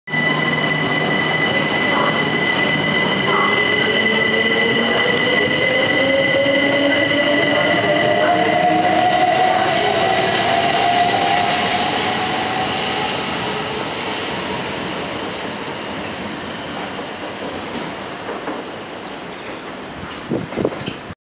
375702 Departs Tonbridge
375702, a Southeastern Class 375 Electrostar is seen here leaving Tonbridge with a service to London Charing Cross. This was taken on Saturday, April 30th 2011.